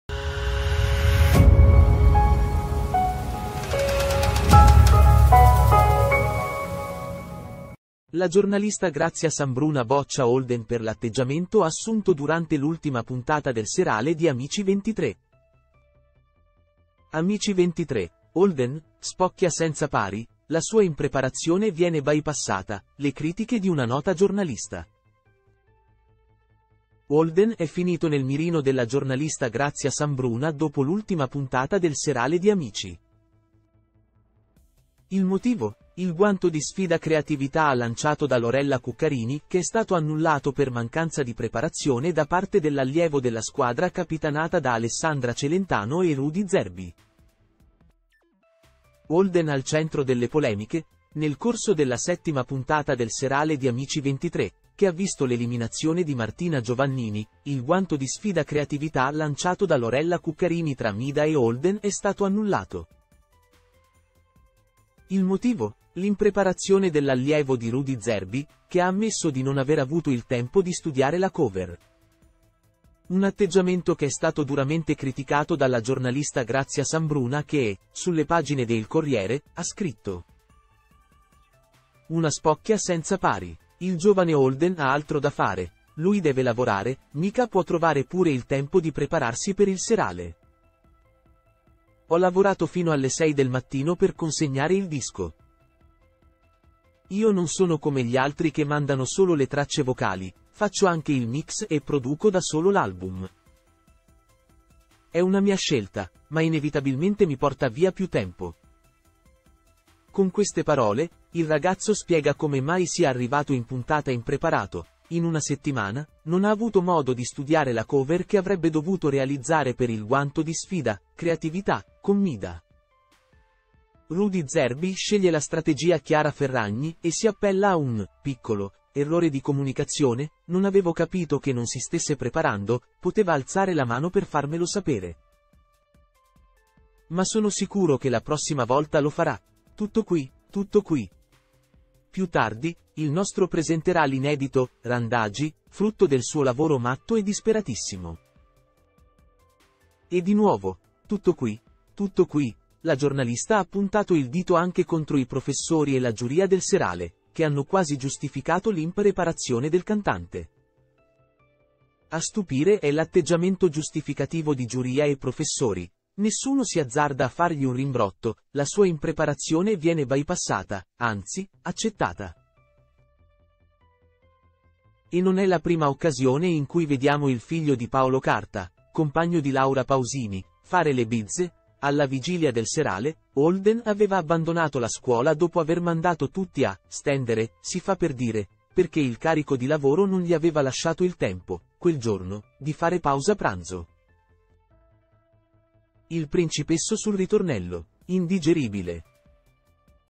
Amici 23 Holden Spocchia senza pari, la sua impreparazione viene bypassata, le critiche di una n
Holden è finito nel mirino della giornalista Grazia Sambruna dopo l'ultima puntata del Serale di Amici. Il motivo? Il guanto di sfida creatività lanciato da Lorella Cuccarini che è stato annullato per mancanza di preparazione da parte dell'allievo della squadra capitanata da Alessandra Celentano e Rudy Zerbi.
Holden al centro delle polemiche